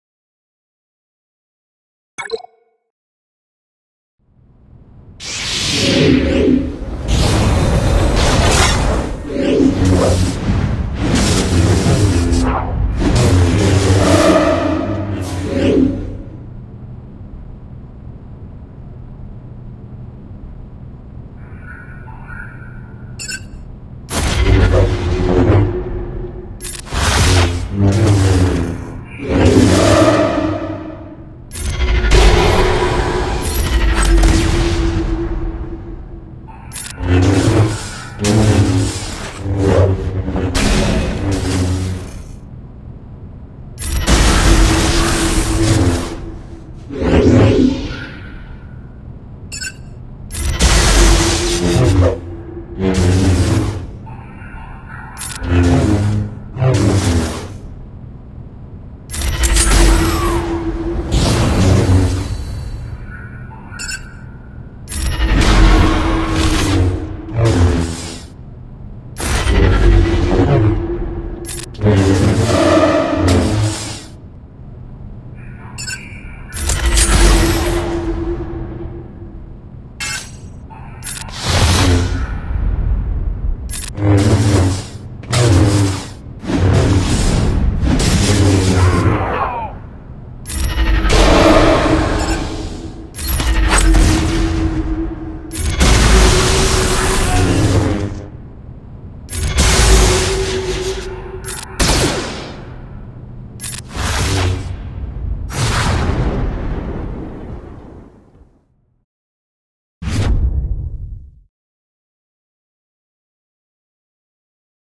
Arena: Sith Empire v Revan Jedi
Am trying different comps and yet to move mods